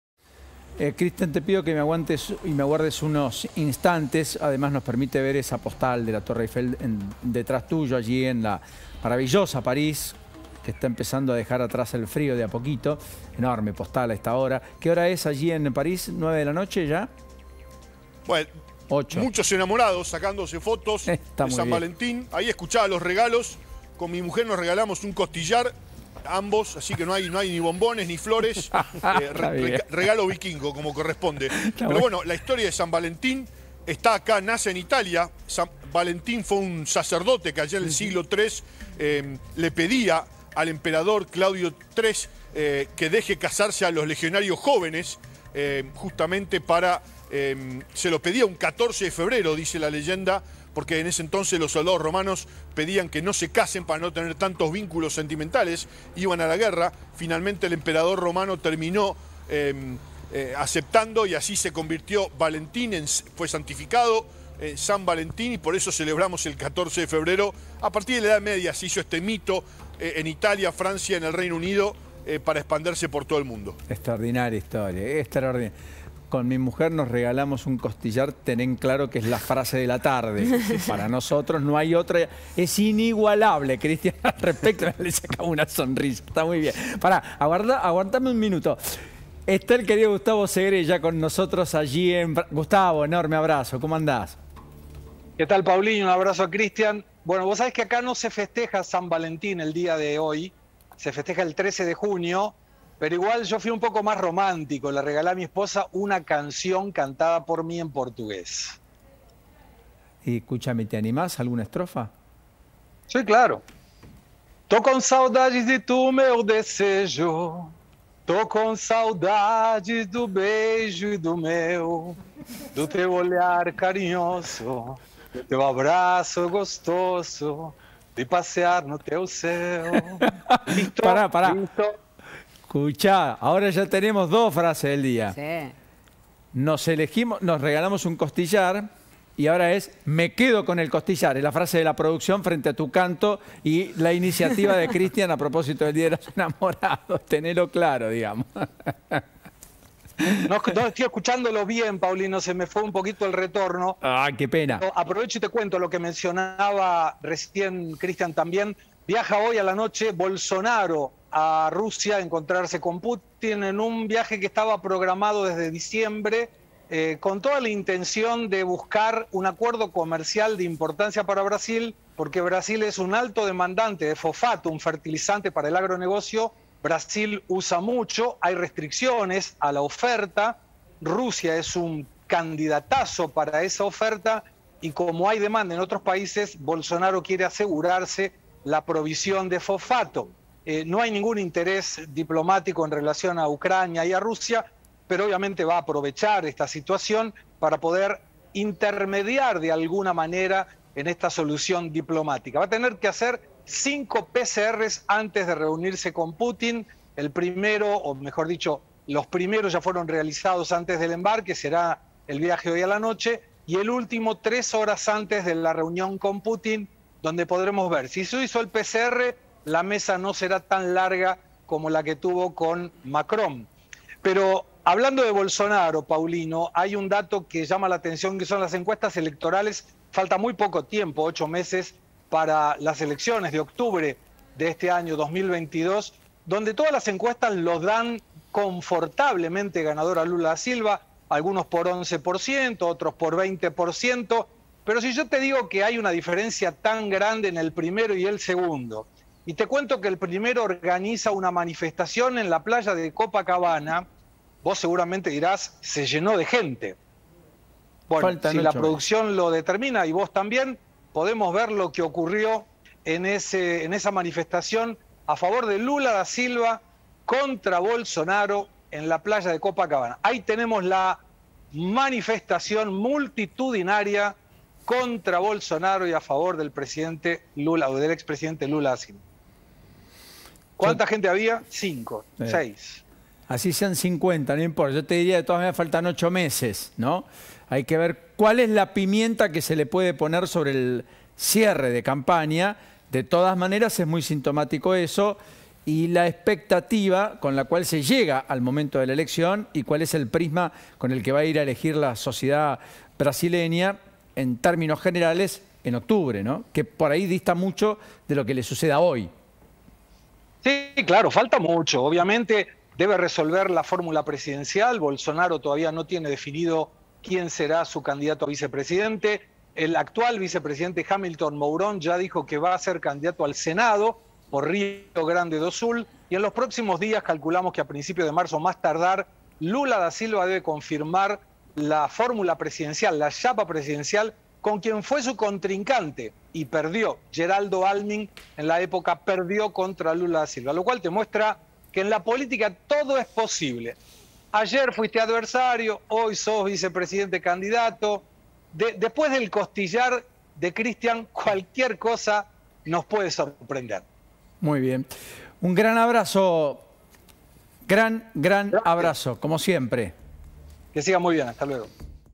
El dia de los enamorados: Los regalos que hicimos con Christian Martin a nuestas esposas (hilario)
El viaje del Pdte Bolsonaro a Rusia, Una manifestacion "multitudinaria" a favor de Lula y pidiendo que Bolsonaro se vaya, entre otros temas

Si miras el video, elegi que regalo te gusto mas: Si el de Christian o el mio